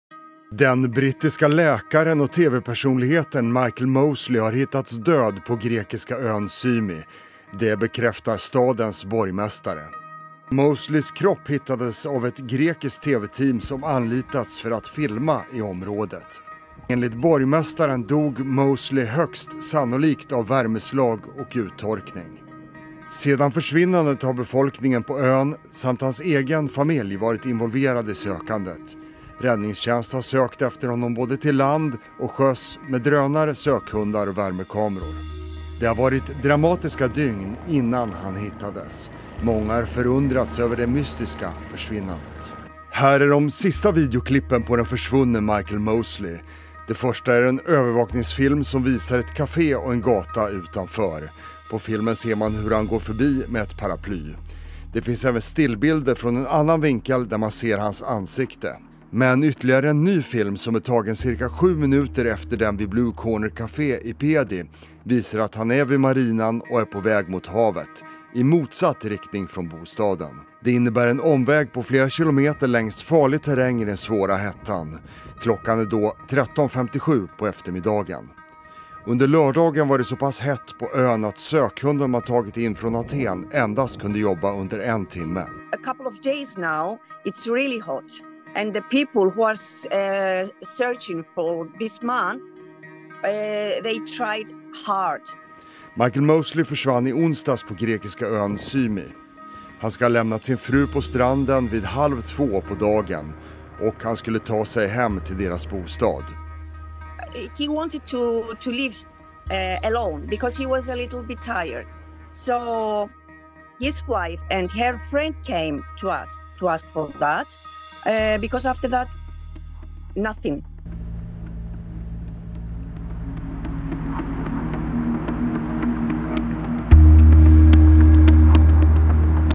Michael Mosley har hittats död i Grekland
Den brittiska läkaren och tv-personligheten Michael Mosley har hittats död på grekiska ön Symi, bekräftar stadens borgmästare. Mosley försvann i onsdags efter att ha gått iväg på en promenad själv.